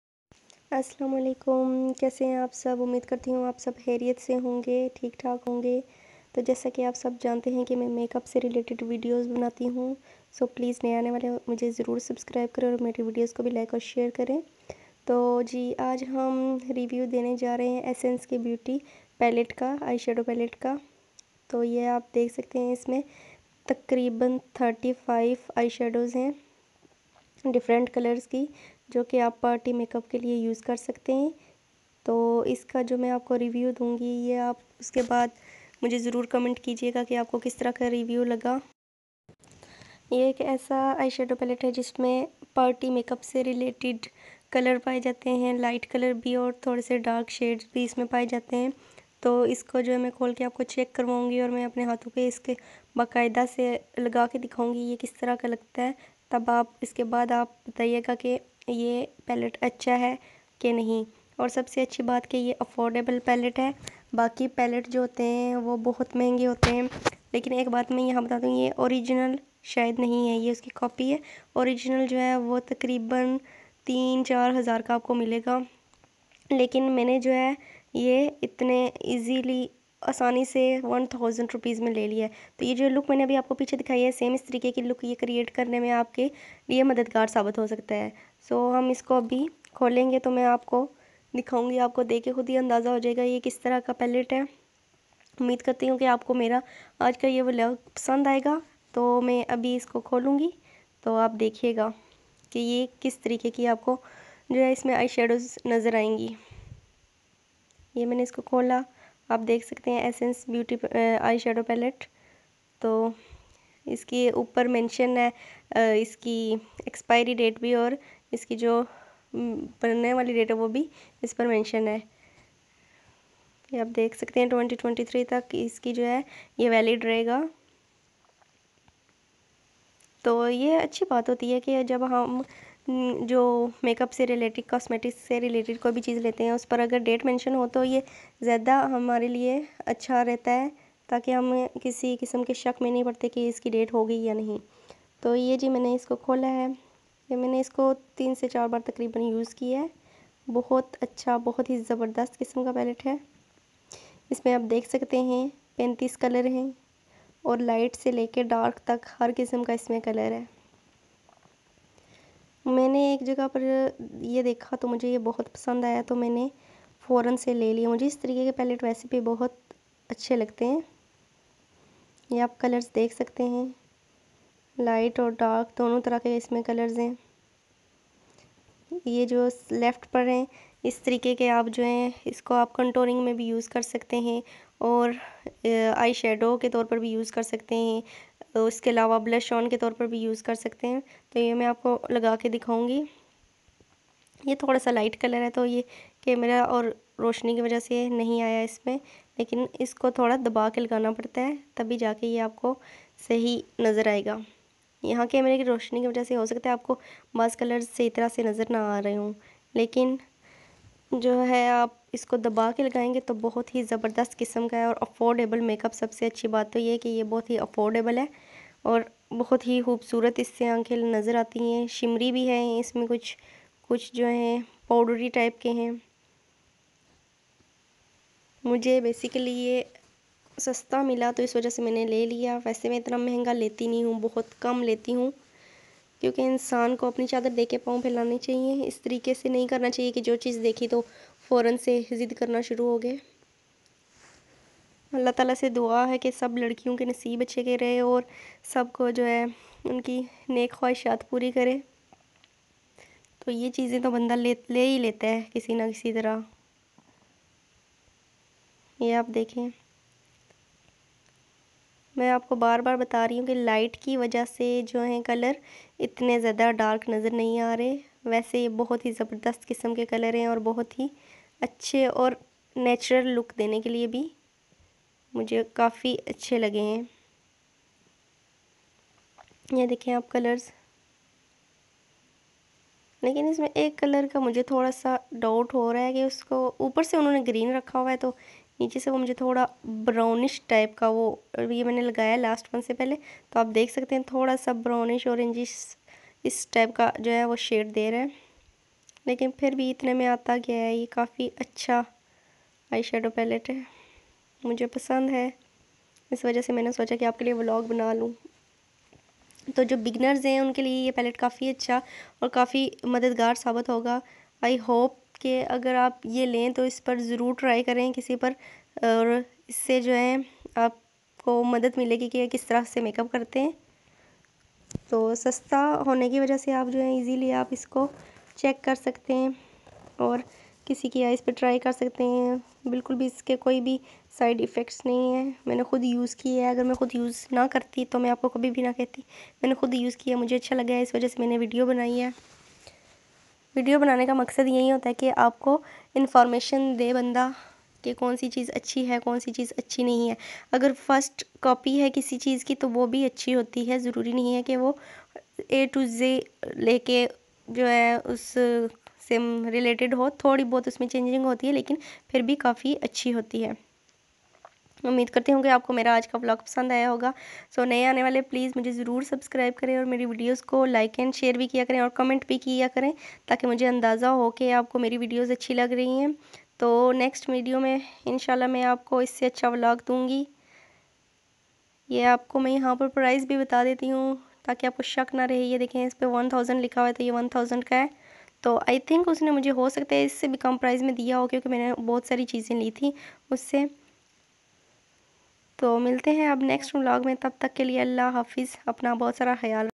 essence eyeshadow palette review | makeup for beginners
Girls yha pr apko affordable makeup sy mutalq video bna kr btati hu.